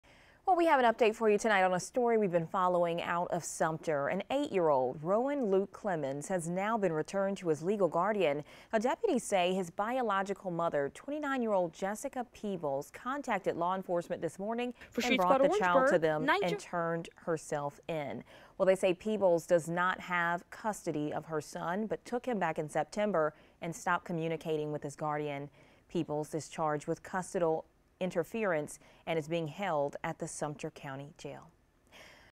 Police: Child returned to guardian after biological mother turns herself in
According to the Sumter County Sheriff's Office, the child was initially taken by his biological mother, Jessica Peebles in September.